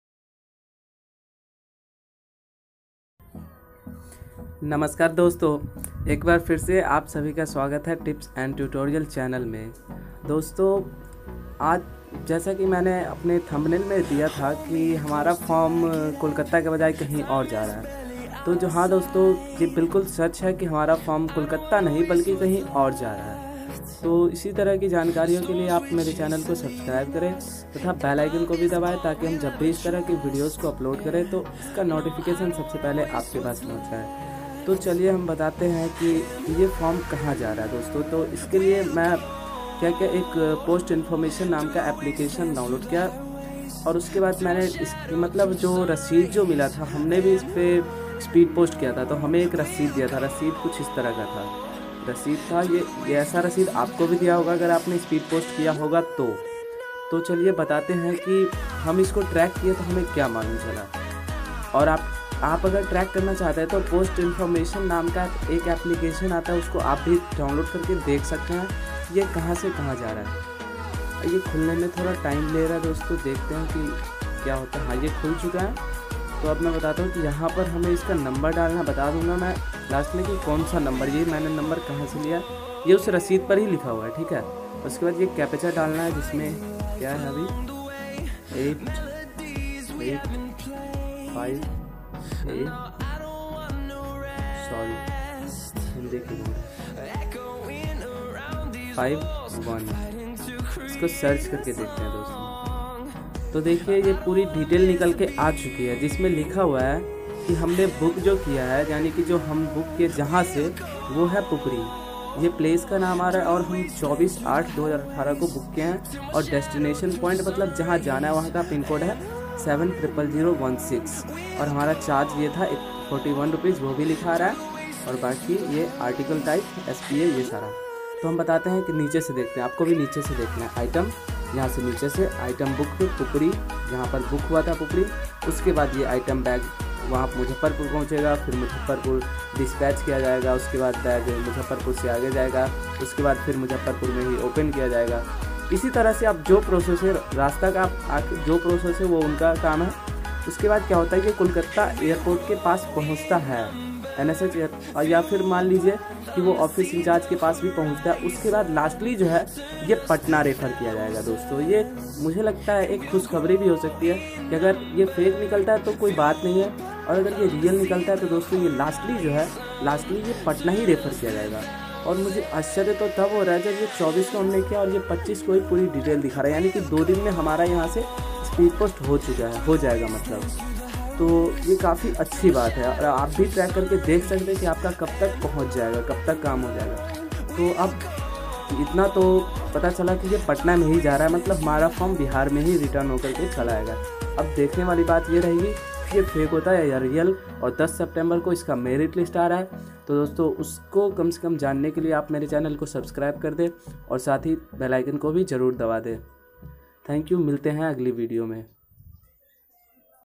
Gramin Kaushal Vikas Mission का Form जा रहा है कहीं और..
10 September ko

sarkariwork.com ki esi ki taisi karenge bhaiyon.

देखिए दोस्तों हम लोगों ने इसका फॉर्म भरा है और यह लगभग लगभग फेक नजर आ रहा है। बात रही कि मैंने यह वीडियो क्यों बनाया दोस्तों तो मान लीजिए कि अगर आपको कोई valuable आदमी आपको कुछ कहें किसी के बारे में तो आपको सरासर वह सही लगेगा ठीक उसी तरह हमने भी sarkari.com के कहने पर इसका स्पीड पोस्ट किया क्योंकि क्योंकि यह वेबसाइट आए दिन अच्छे-अच्छे नौकरी और सच्ची नौकरी को लाते रहते हैं।
और अगर यह झूठ निकलता है तो इसमें सबसे बड़ी गलती इस वेबसाइट की है क्योंकि एक भूखा आदमी अपने खाने के लिए कुछ भी कर सकता है ठीक उसी तरह एक बेरोजगार आदमी भी रोजगार के लिए करने को तैयार रहता है।
तो हमें इस वेबसाइट के खिलाफ कुछ करना चाहिए या नहीं आप मुझे रिप्लाई दे कर बता सकते हैं।
धन्यवाद.